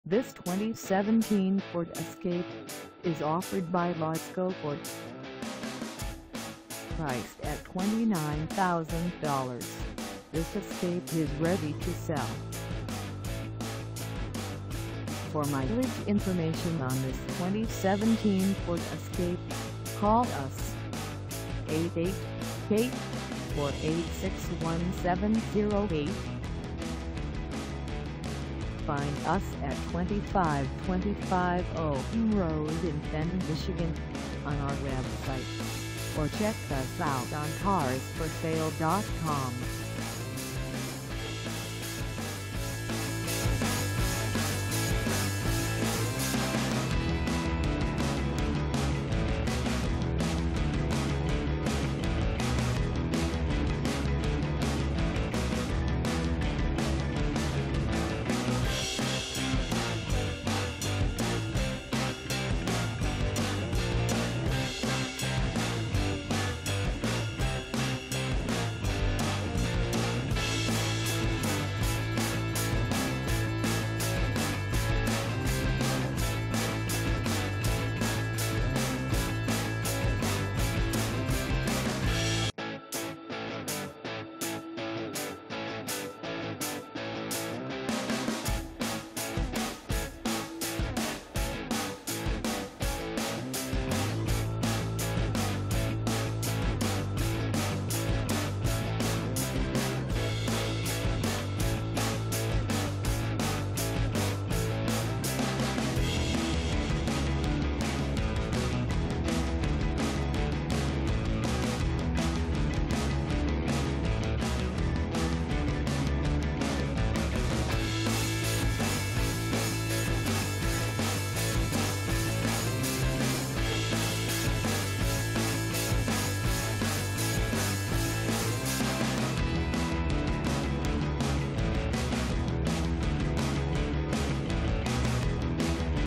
2017 Ford Escape Titanium 4dr SUV for sale in FENTON, MI 484
This 2017 Ford Escape Titanium 4dr SUV is for sale in FENTON, MI 48430 at LASCO FORD.

Engine: 2.0L I4
Transmission: Automatic 6-Speed
Color: White Platinum Metallic Tri Co
Mileage: 0
Contact LASCO FORD 2525 OWEN RD FENTON, MI 48430 to test drive this 2017 Ford Escape Titanium 4dr SUV today.